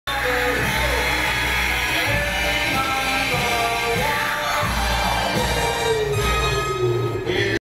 Models walk the runway at Betsey Johnson - Spring 2014 Me...
Models walk the runway at Betsey Johnson - Spring 2014 Mercedes-Benz Fashion Week in New York, NY, on 9/11/13.